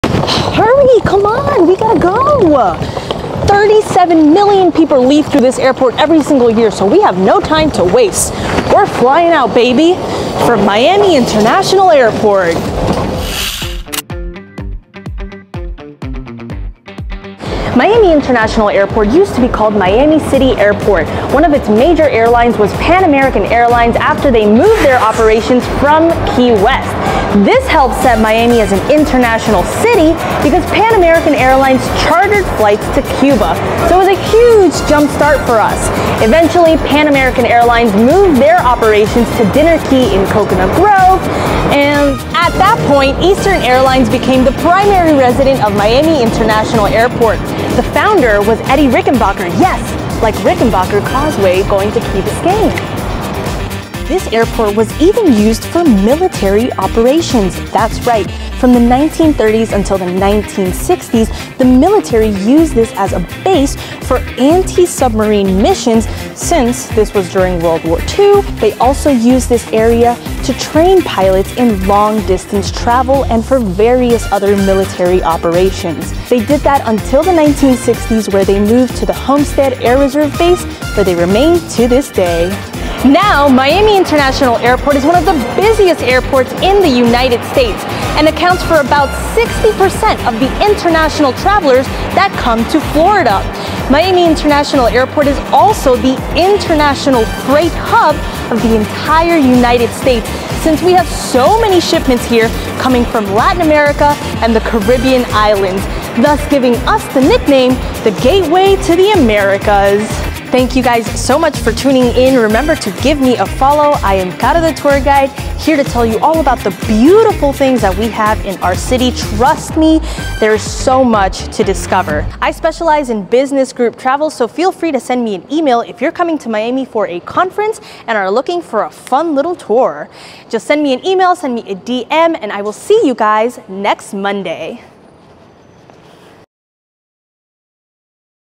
Miami International Airport
Today let’s visit one of the largest economic engines for @miamidadecounty ! - @iflymia

✨Miami City Airport was opened in 1928✨

📦 It has grown into an astounding institution, offering more than 90 air carriers and is the top U.S. airport for international freight 📦

Fast Facts:
✈️ offers more flights to Latin America than any other airport in the U.S.
✈️ second-busiest airport for international passengers
✈️ Pan-Am Airways was one of the primary airlines to use the space
✈️ Used as a military base during WWII

Did you catch the hidden fun fact about the Rickenbacker Causeway?? (This is the bridge that leads to Key Biscayne)

*Caro the Tour Guide is a professional tour guide in Miami, specializing in group travel; whether it’s for leisure, business, or adventure.